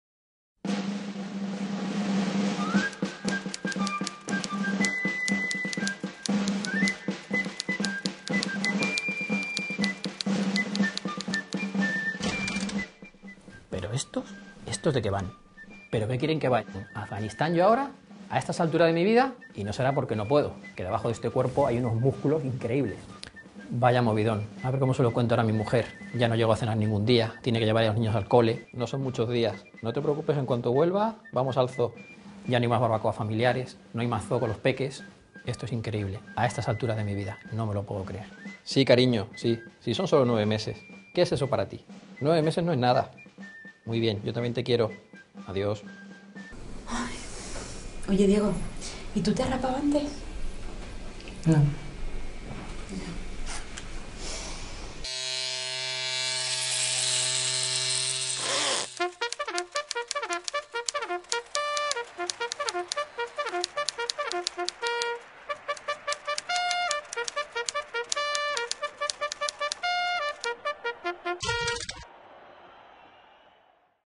Vuelve el servicio militar, la reacción nº 5
Reacciones de Fran al recibir la notificación de incorporarse a filas, se acabó para él vestir de traje y corbata todas las mañanas, sus 2 horas diarias en el gimnasio y las barbacoas familiares. Más info